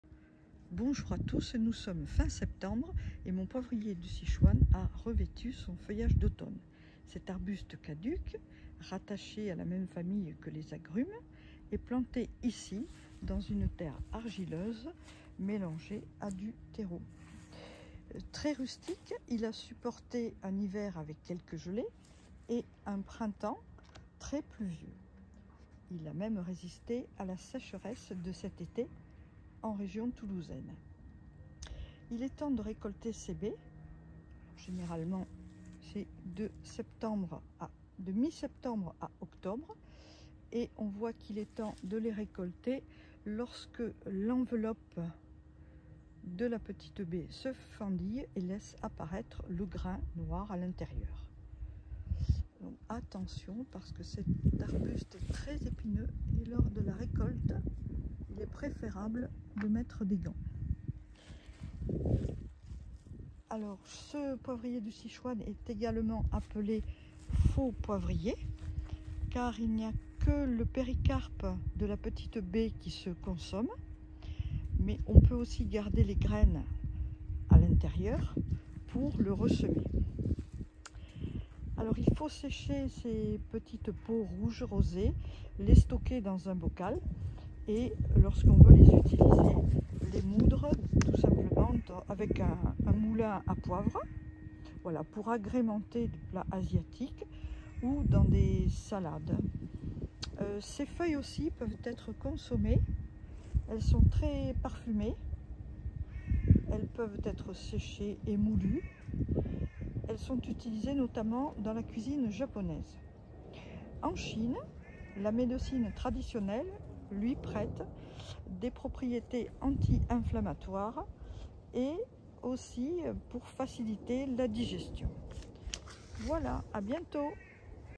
Quand récolter poivrier du Sichuan ?
@helene redon
Fin septembre , on peut commencer à récolter les baies du  poivrier du Sichuan.  Il est couvert de petites baies rouges dont  on ne consomme que l'enveloppe . Le goût est surprenant , légèrement citronné , frais et poivré.